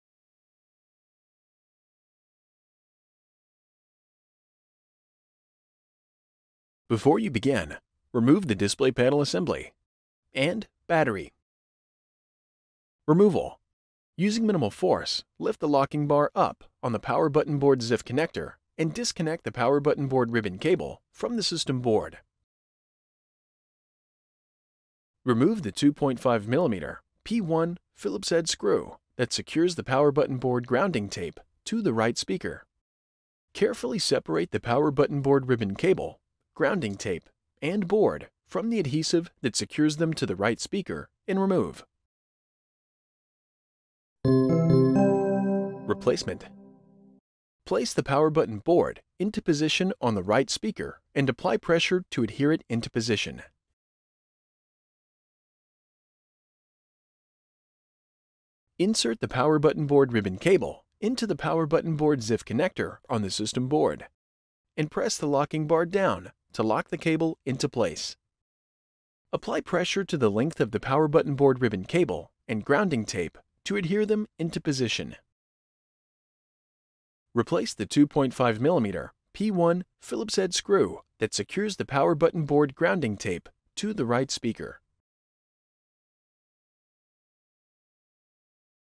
Remove and Replace the Power Button Board | HP ENVY x2
Learn how to remove and replace the power button board on the HP ENVY x2.